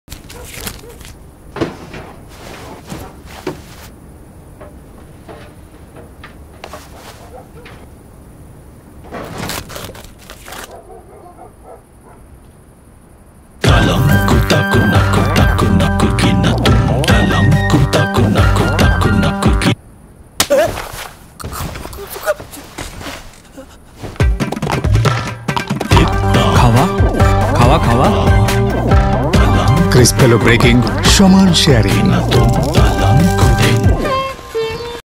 Cadbury Dairy Milk Crispello - Can’t steal this - 35sec Bengali
Crispello breaking, barabar sharing.
It doesn’t matter if you are a newborn, teenager, thief, teacher or a saint. You always share a Crispello equally. Always.